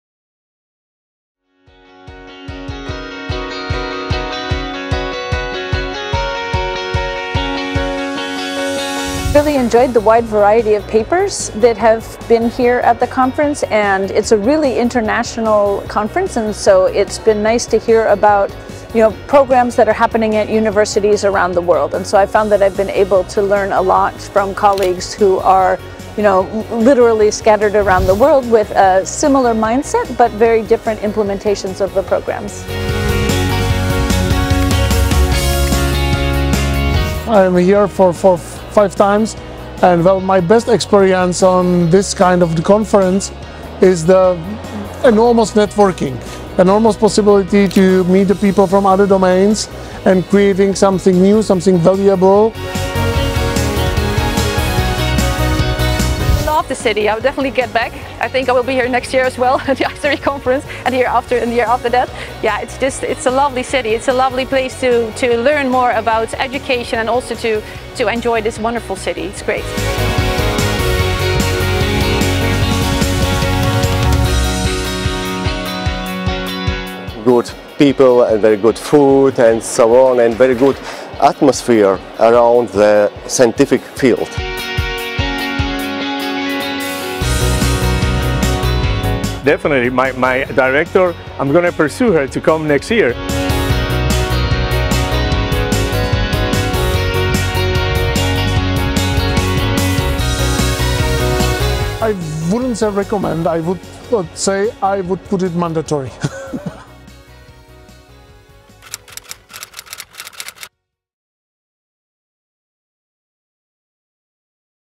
ICERI2022 Aftermovie
Thank you for participating in the 15th edition of the conference, ICERI2022!
We hope that you were able to learn about a range of perspectives and gain an insight into the valuable research of our attendees.